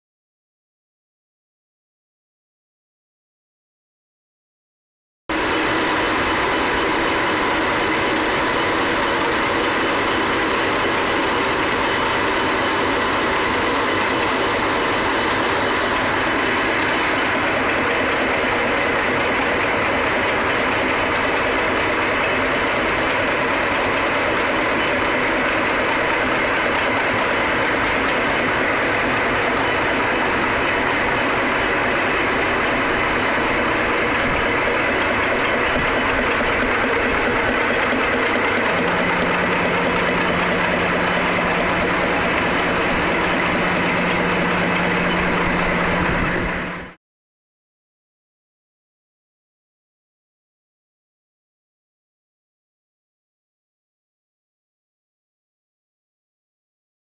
Пресс для производства С образных скоб
Предлагаем вашему вниманию видео работы пресса для изготовления скоб. С полным описанием и характеристиками вы можете ознакомиться, посетив наш сайт